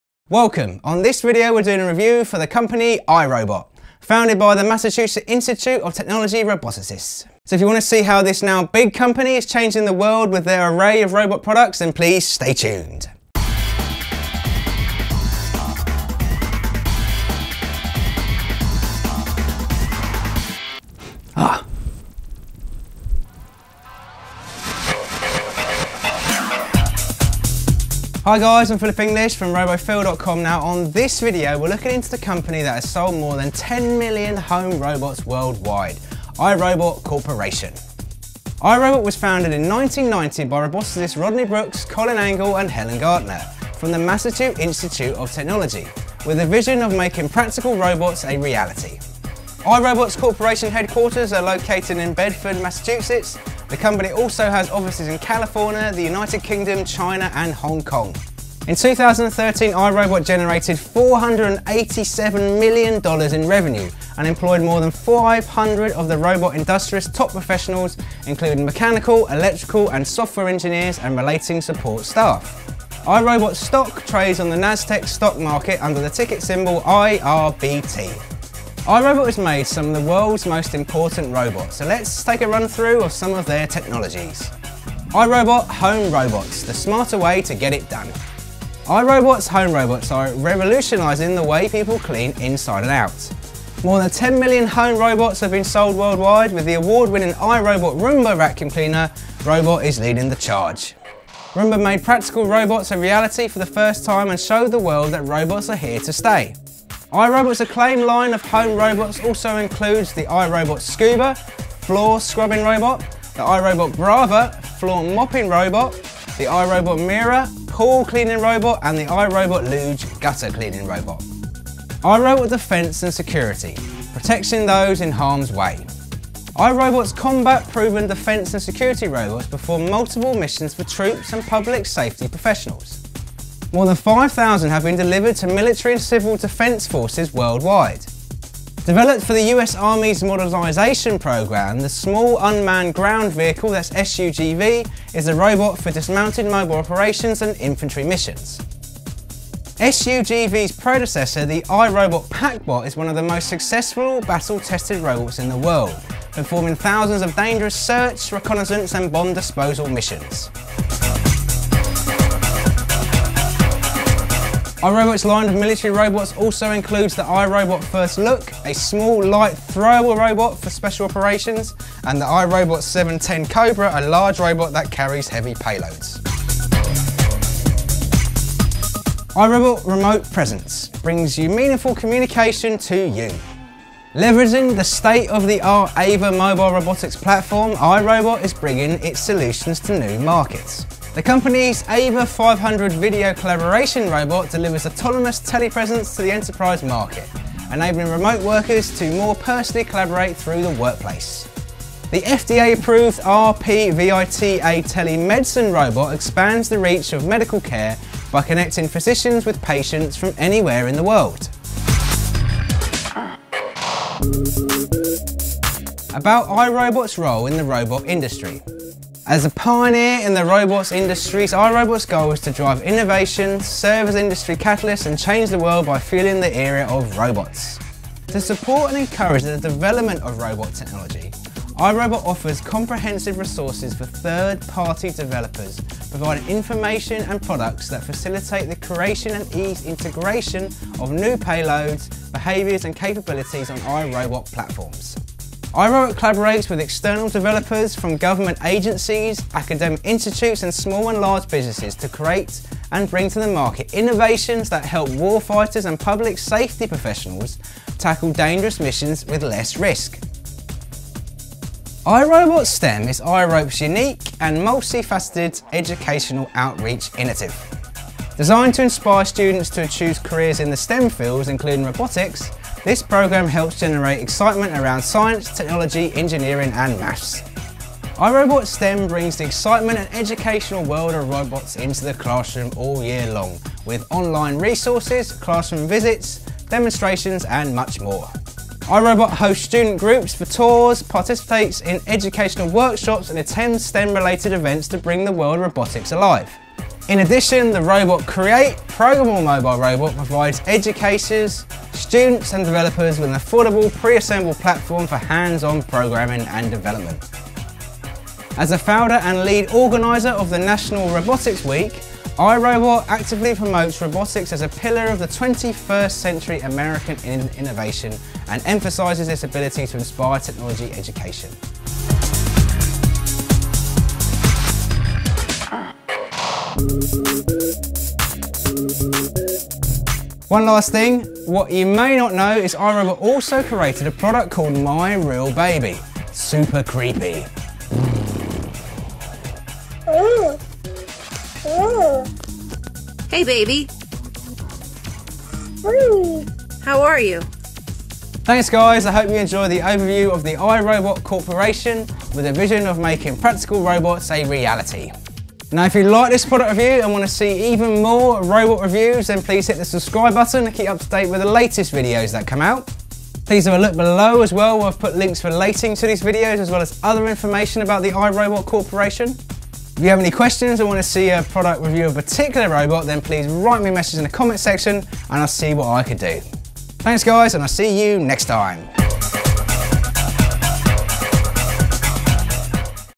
iRobot Corporation Review
Welcome, and on this video we are doing review for the Company Irobot, founded by the Massachusetts Institute of Technology oboticists!

Hi Guys, I’m Philip English from robophil.com, now on this video we are looking in to the company that has sold more than 10 million home robots worldwide, iRobot Corporation!

iRobot was founded in 1990 by robotics Rodney Brooks, Colin Angle & Helen Greiner from the Massachusetts Institute of Technology, with the vision of making practical robots a reality. iRobot’s corporate headquarters are located in Bedford, Mass. The company also has offices in California, the United Kingdom, China and
Hong Kong. In 2013, iRobot generated $487 million in revenue and employed more than 500 of the robot industry’s top professionals, including mechanical, electrical and software engineers and related support staff. iRobot stock trades on the NASDAQ stock market under the ticker symbol IRBT. iRobot has made some of the world’s most important robots. So lets have a run through of there technology’s: iRobot Home Robots: The smarter way to get it done – iRobot’s home robots are revolutionizing the way people clean – inside and out. More than 10 million home robots have been sold worldwide. The award-winning iRobot Roomba® Vacuum Cleaning Robot is leading the charge. Roomba made practical robots a reality for the first time and showed the world that robots are here to stay.iRobot’s acclaimed line of home robots also includes the iRobot Scooba® Floor Scrubbing Robot, the iRobot Braava™ Floor Mopping Robot, the iRobot Mirra Pool Cleaning Robot and the iRobot Looj® Gutter Cleaning Robot. iRobot Defense and Security: Protecting those in harm’s way iRobot’s combat-proven defense and security robots perform multiple missions for troops and public safety professionals. More than 5,000 have been delivered to military and civil defense forces worldwide. Developed for the U.S. Army’s modernization program, Small Unmanned Ground Vehicle (SUGV) is the robot for dismounted mobile operations and infantry missions. SUGV’s predecessor, the iRobot PackBot®, is one of the most successful battle-tested robots in the world, performing thousands of dangerous search, reconnaissance and bomb-disposal missions. iRobot’s line of military robots also includes the iRobot FirstLook®, a small, light, throwable robot for special operations, and the iRobot® 710 Kobra, a large robot that carries heavy payloads. iRobot Remote Presence: Brings meaningful communication to you Leveraging its state-of-the-art Ava® mobile robotics platform, iRobot is bringing its solutions to new markets. The company’s Ava® 500 video collaboration robot delivers autonomous telepresence to the enterprise market, enabling remote workers to more personally collaborate throughout the workplace. The FDA-approved RP-VITA™ telemedicine robot expands the reach of medical care by connecting physicians with patients from anywhere in the world. About iRobot Role In The Robot Industry as a pioneer in the robot industry, iRobot’s goal is to drive innovation, serve as an industry catalyst and change the world by fueling the era of robots. To support and encourage the development of robot technology, iRobot offers comprehensive resources for third-party developers, providing information and products that facilitate the creation and easy integration of new payloads, behaviors and capabilities on iRobot platforms.